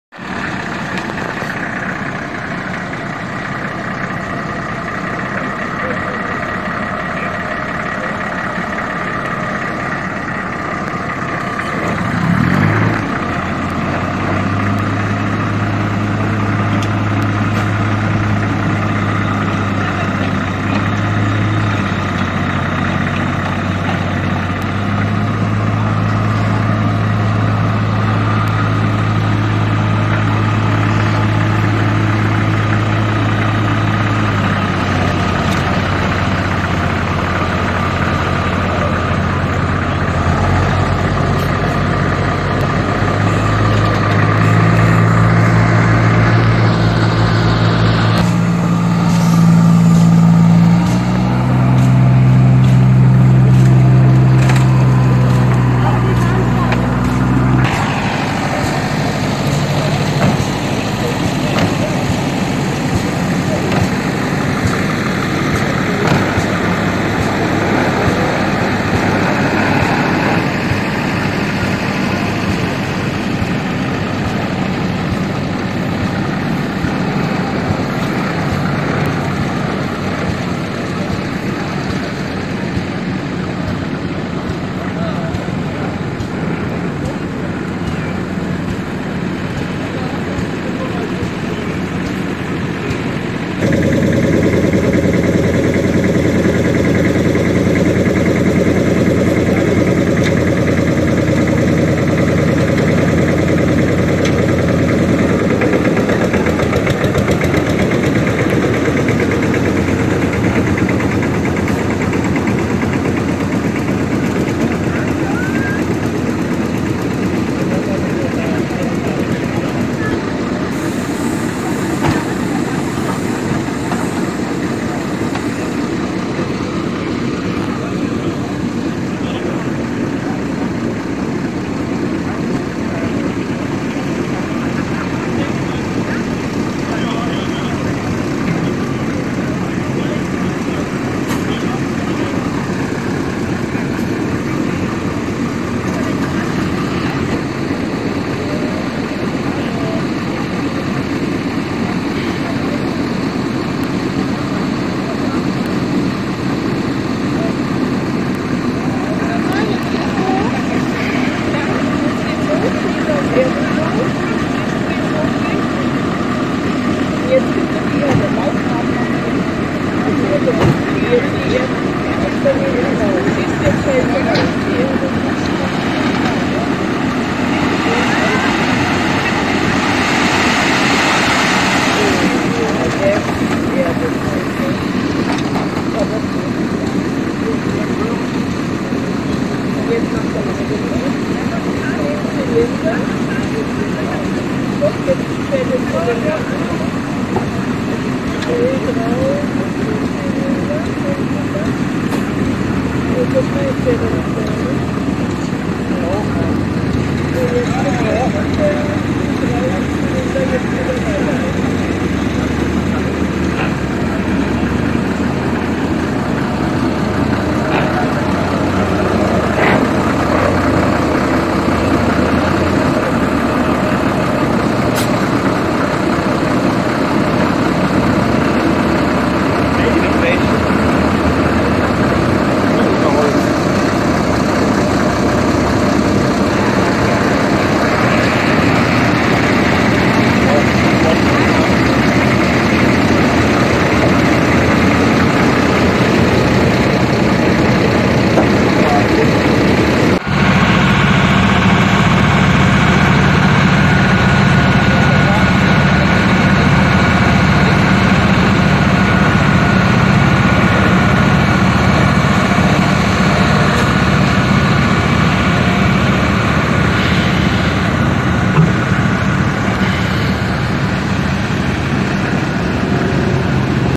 Borgward Kipper | Patina | Wirtschaftswunder live erleben | Historische Baustelle Huttenheim 2024
Auf der historischen Baustelle in Huttenheim war auch dieses Jahr wieder jede Menge los. So gab es einige Kipper zu bestaunen, welche von fleißigen Baggern beladen wurde. Besonders heraus stach dieser Borgward, welcher mit einer tollen original Patina von seinem langen Arbeitsleben erzählt. Wirklich ein wunderschönes Fahrzeug in seinem natürlichen Lebensumfeld!